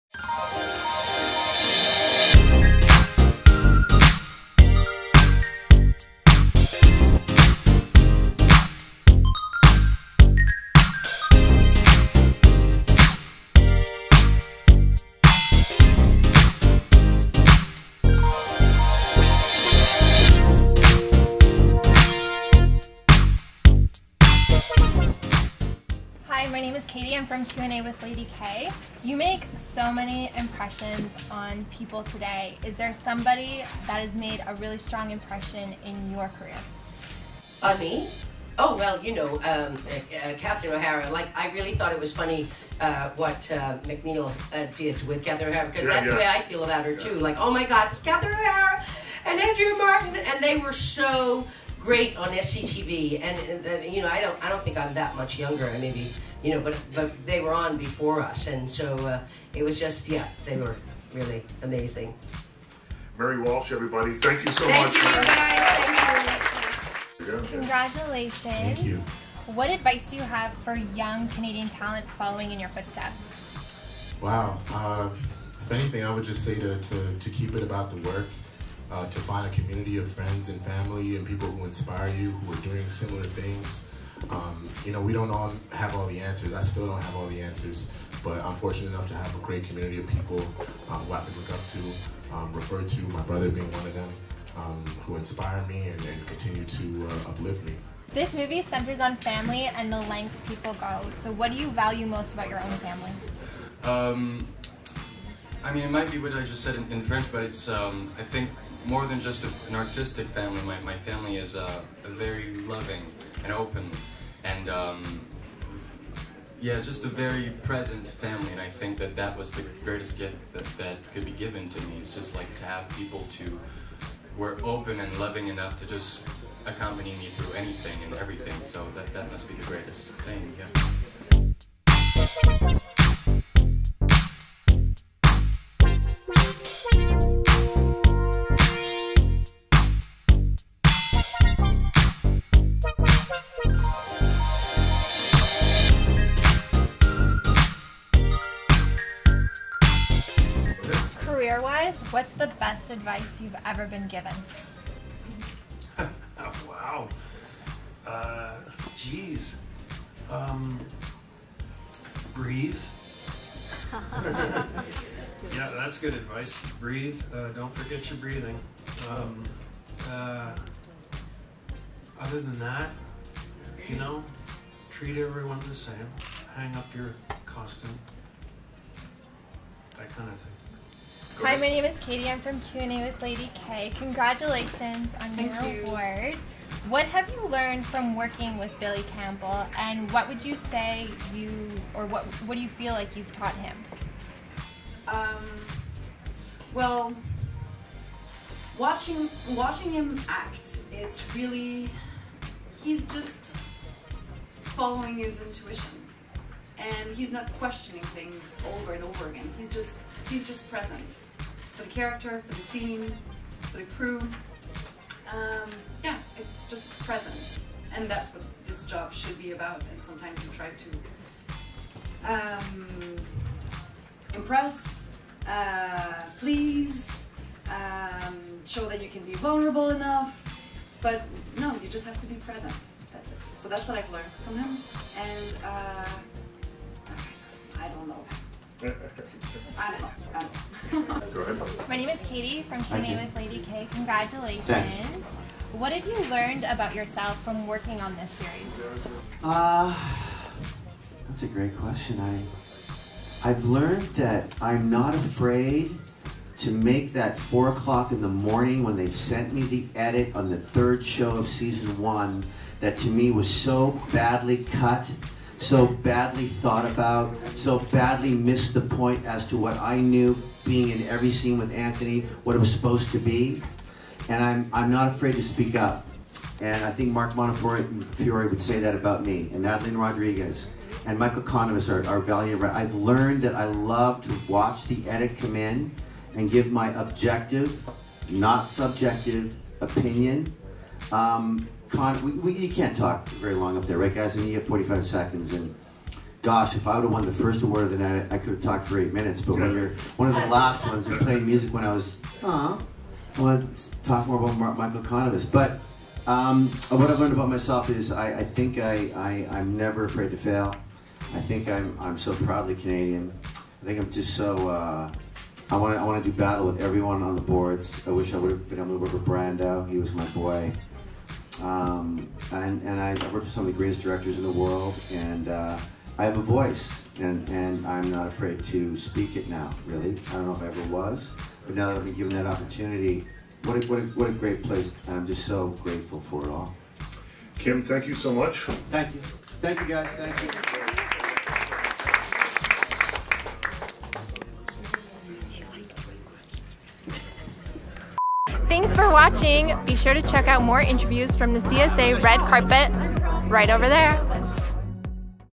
Q&A with Canadian Screen Awards Winners
I had the amazing opportunity to ask some of this year's CSA winners some questions in the press room.

Producers: Katie Favrin & Leah Rifkin
Filmed by Leah Rifkin
Location Audio by Allegra Klein
Edited by Leah Rifkin

Equipment Provided by Scarlet Lens Productions